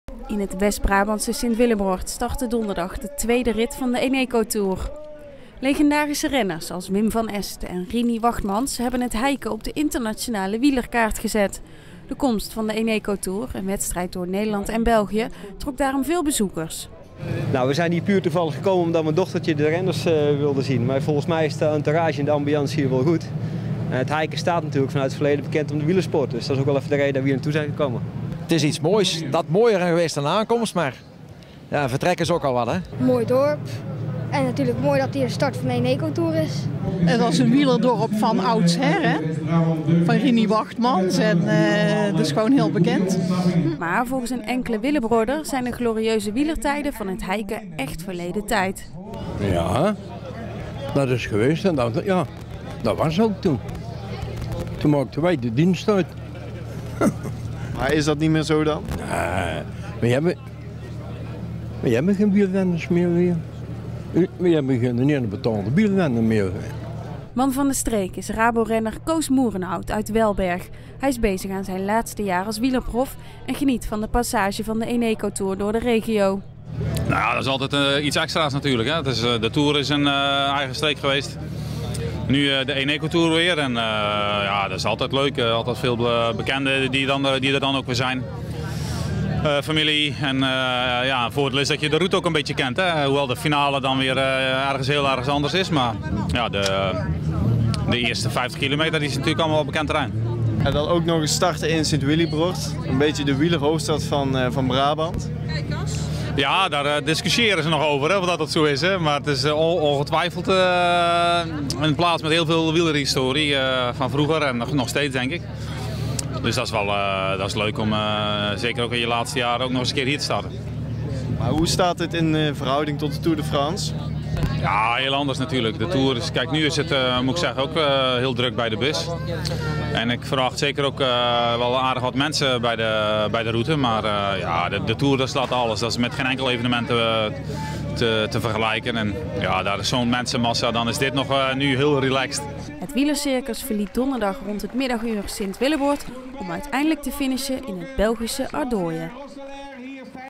Eneco Tour trok door West-Brabant (2)
SINT WILLEBRORD - De tweede etappe van de Eneco Tour vertrok vandaag in Sint Willebrord.